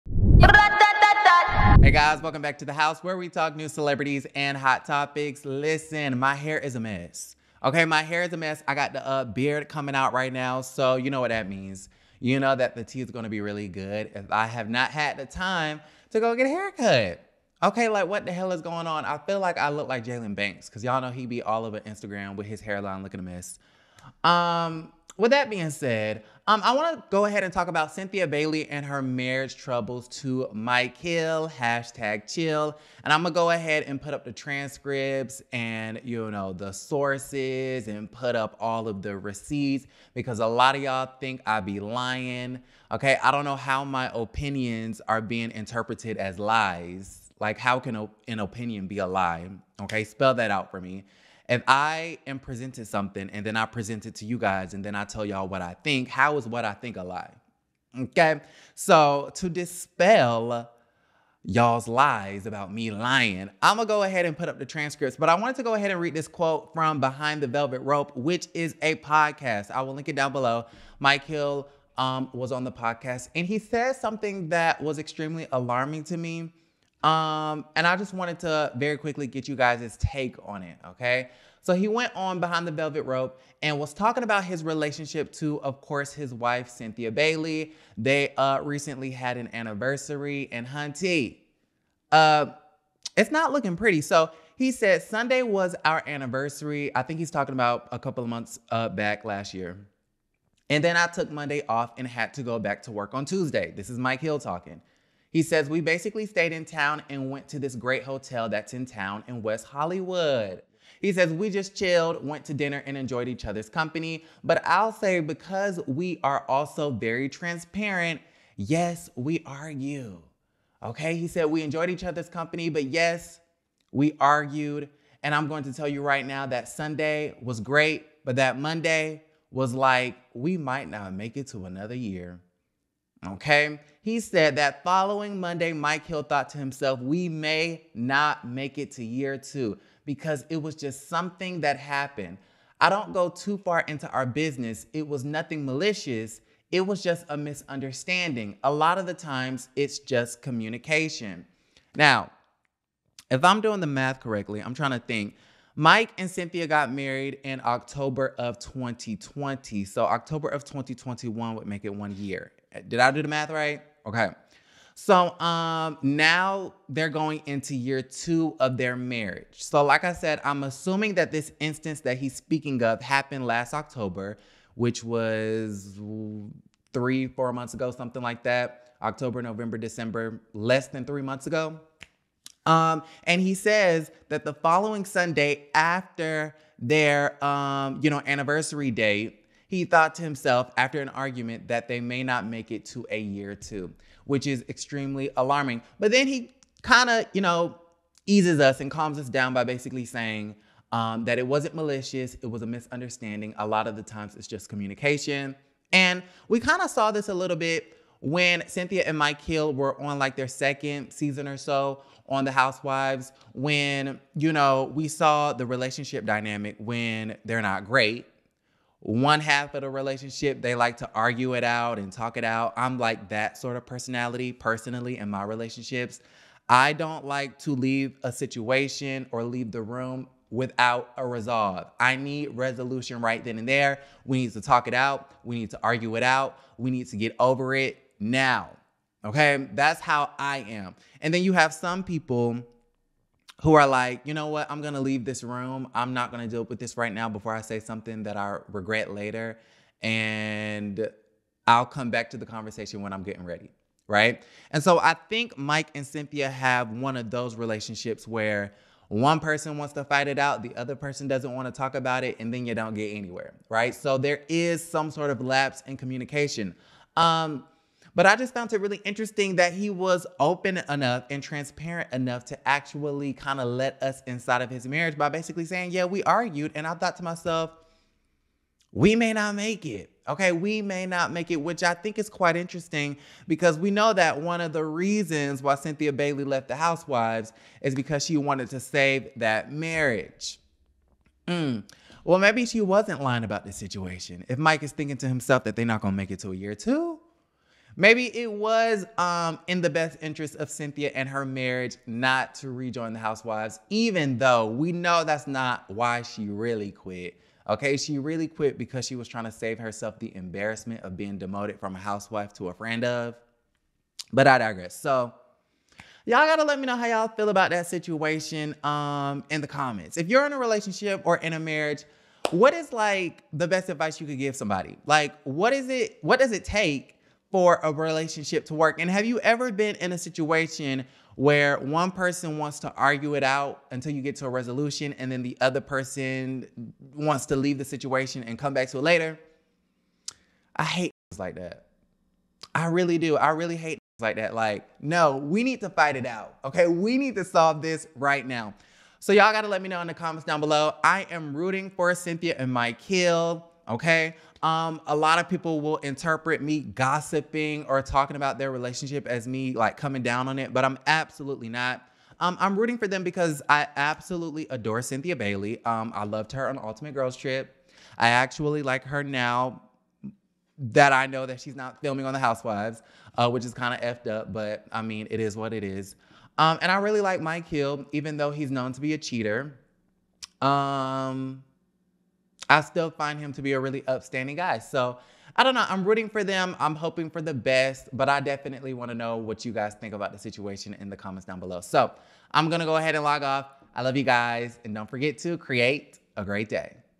Cynthia Bailey MARRIAGE TROUBLES, Mike Hinting Divorce!
Mike Hill discusses marriage issues with Cynthia Bailey.

$aaronekeenan